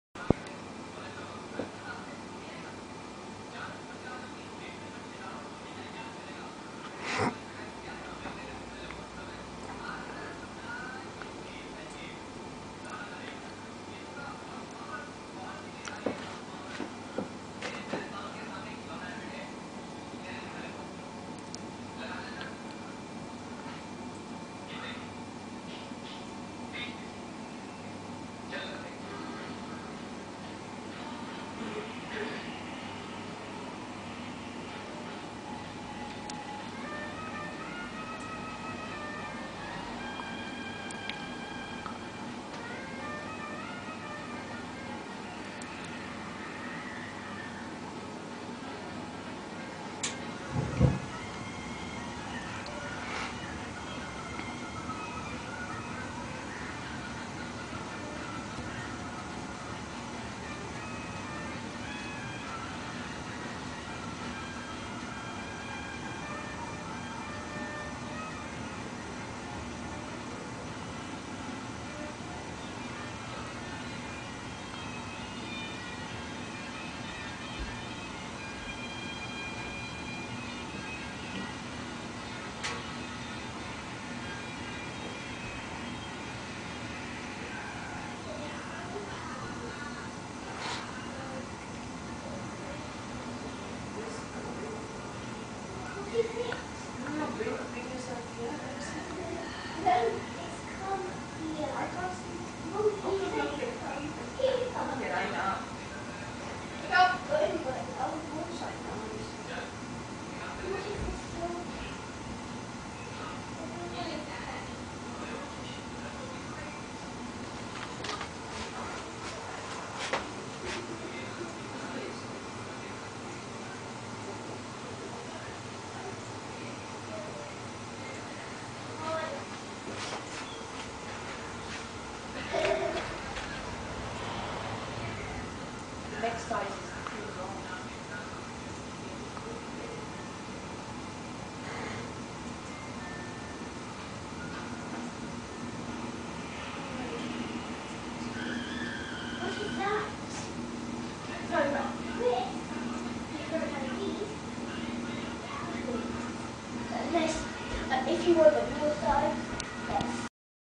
Crambo Visuales muestra uno de los múltiples usos del sistema de DS de Dise
En este video se muestra el uso de una triple pantalla usando el sistema de Digital Signage de Dise en BETT 2009